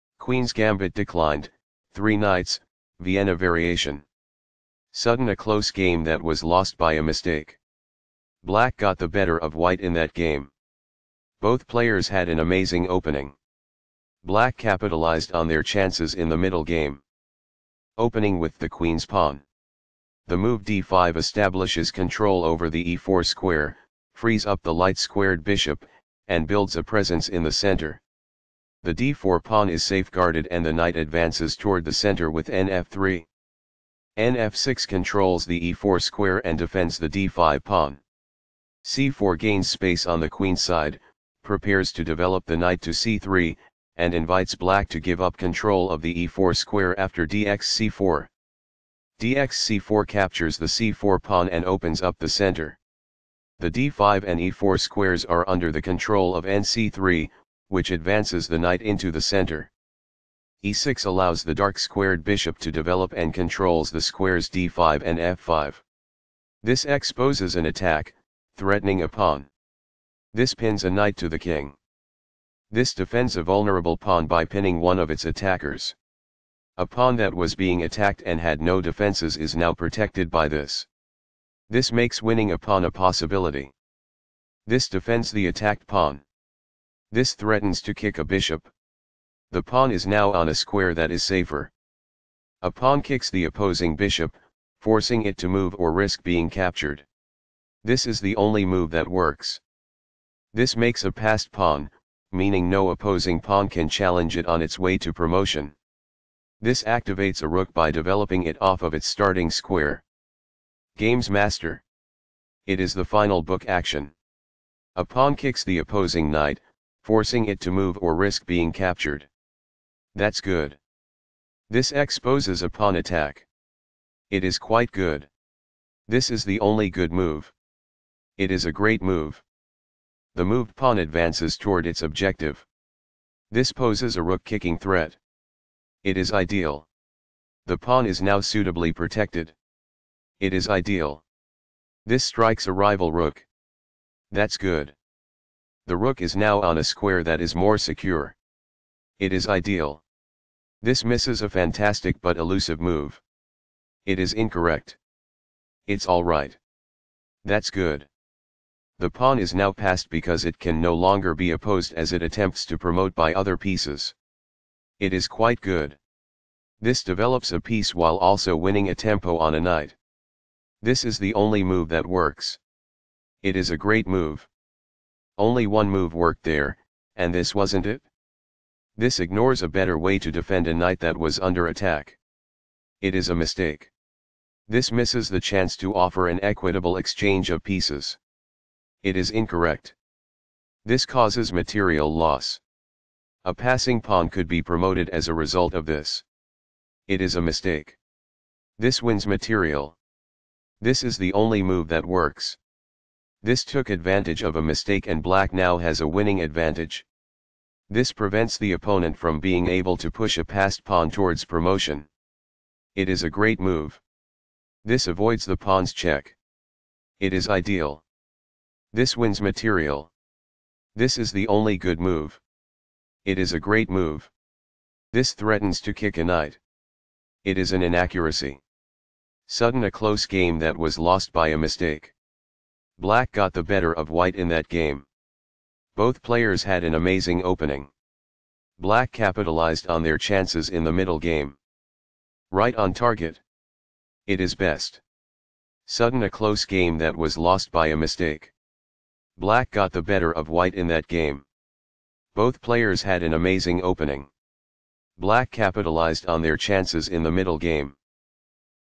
White Bareev Evgeny (RUS), Black Yakovich Yuri (RUS),Queen's Gambit Declined: Three Knights, Vienna
Queen's Gambit Declined: Three Knights, Vienna Variation, Event Spartakiada URS, Site Minsk (Belarus), Date 1986, Round 5, White Bareev Evgeny (RUS), Black Yakovich Yuri (RUS), Result 1/2-1/2, ECO D44, WhiteElo 2470, BlackElo 2440, Remark